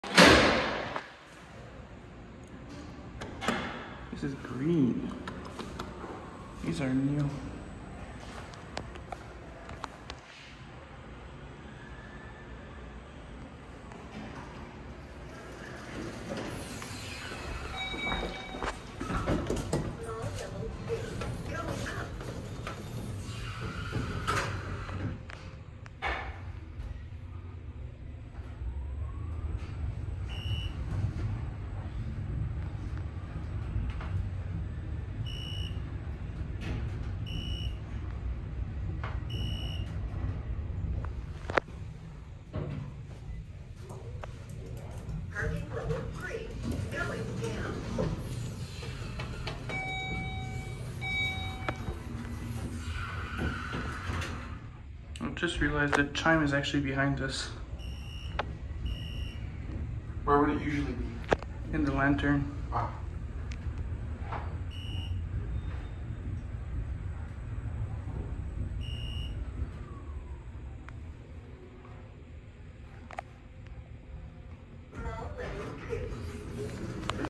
Otis elevators @ CF Toronto Eaton Centre, Downtown Toronto
Brand: Otis

Model: Gen2?

Buttons: DMG/MAD BS California

Drive type: MRL traction

Location: CF Toronto Eaton Centre, Downtown Toronto, Ontario, Canada